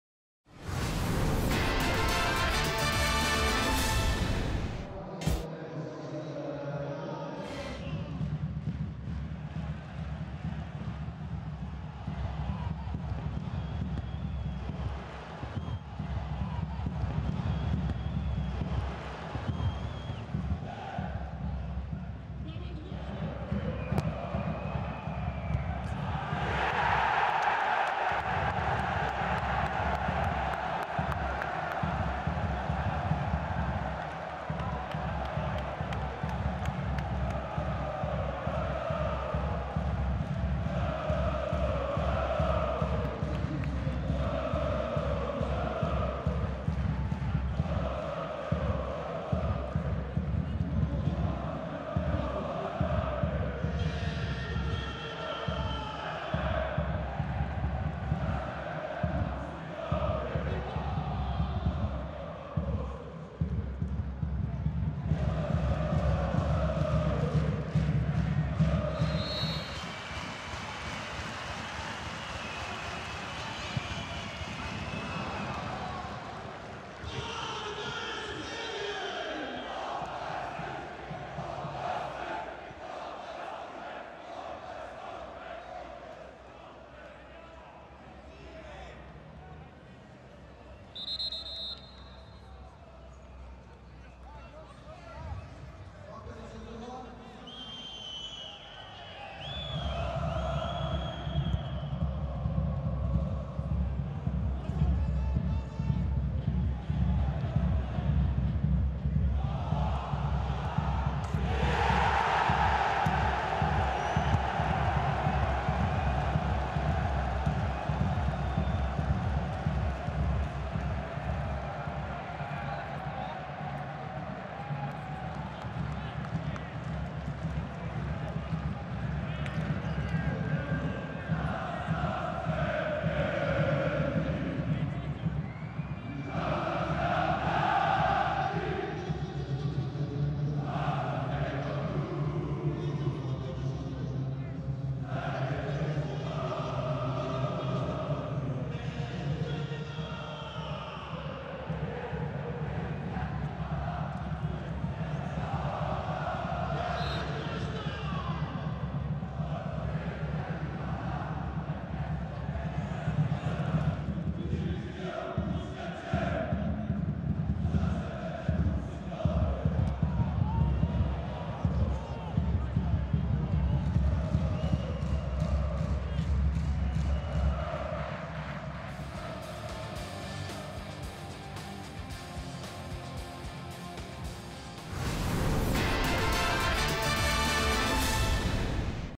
Podbeskidzie Bielsko-Biała - Ruch Chorzów 0:2 | SKRÓT | Ekstraklasa 2014/15 | 33. Kolejka
Podbeskidzie Bielsko-Biała 0:2 Ruch Chorzów
0:1 - Rafał Grodzicki (52')
0:2 - Rołand Gigołajew (61')

EKSTRAKLASA TV ► Mecze na żywo za granicą